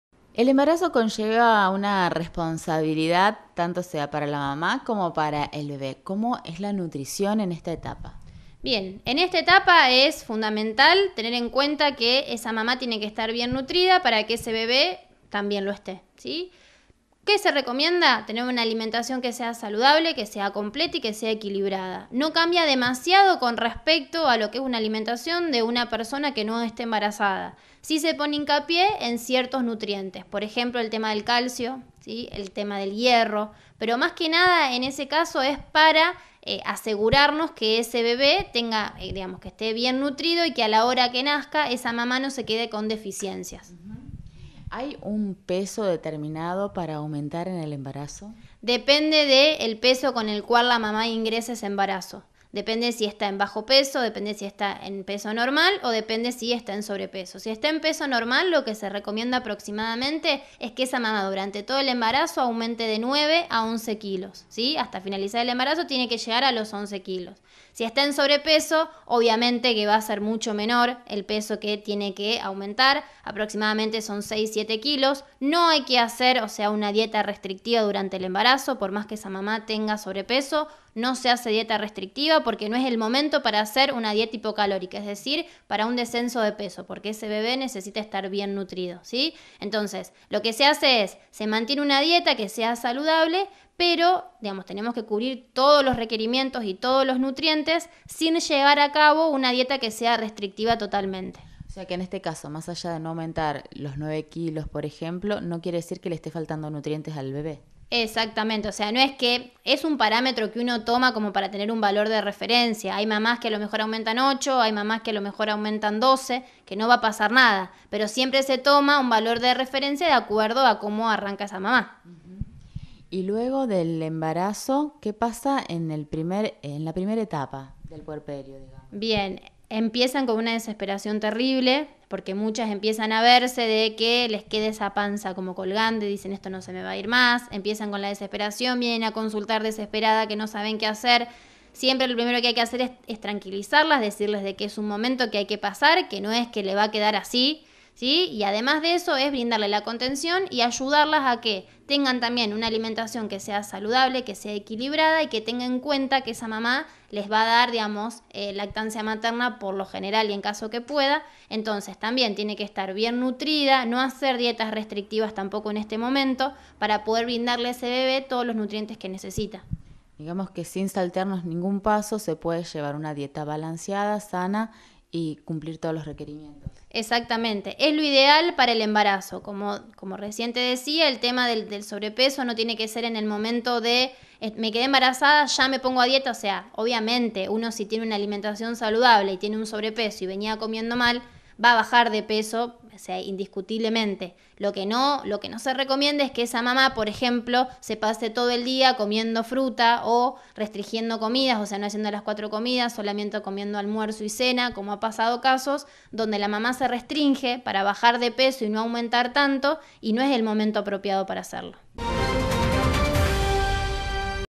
Nutrición: alimentación en el embarazo
Algunas recomendaciones de la Lic. Melina Sassaroli para tener una dieta saludable durante el embarazo.